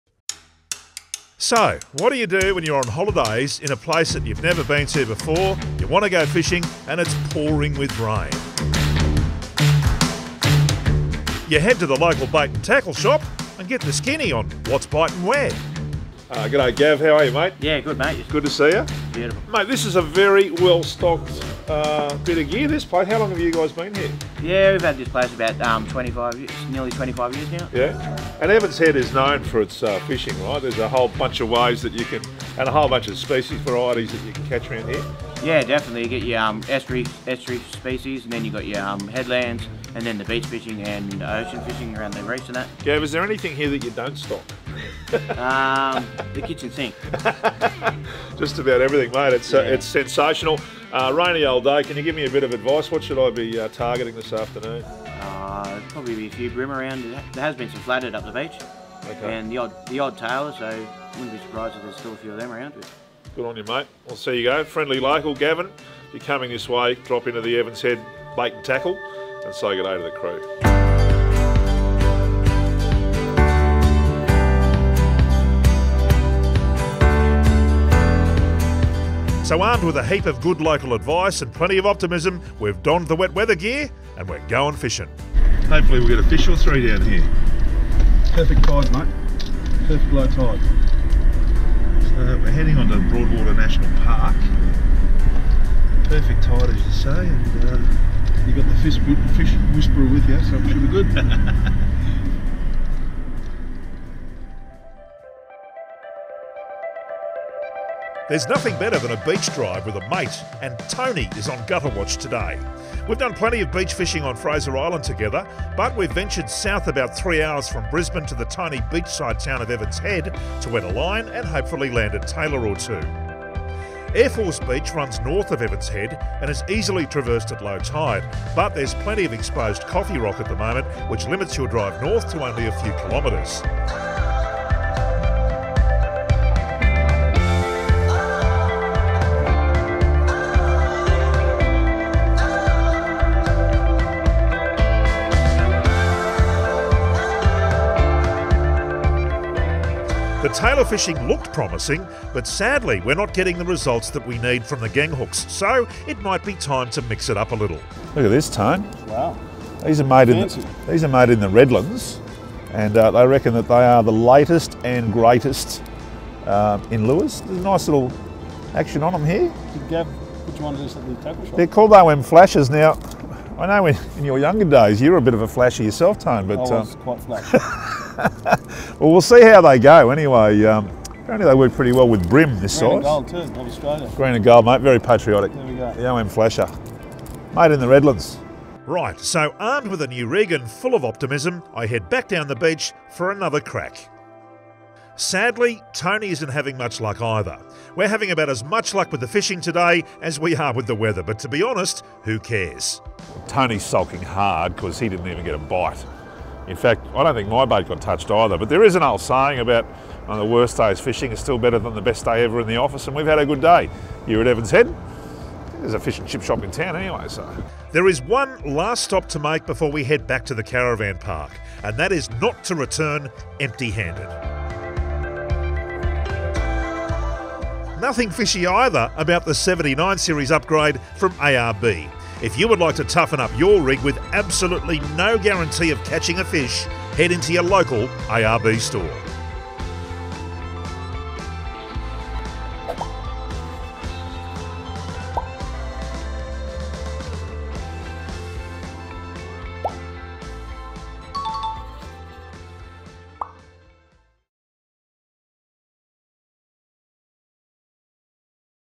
Airforce Beach
We tackle Airforce Beach in a 79 Series with all the trimmings from ARB 4x4 Accessories. This is one of the most popular beaches on the north coast due to its long coastline stretching almost to Ballina.

Toughen up your 4WD at your nearest ARB 4x4 Accessories store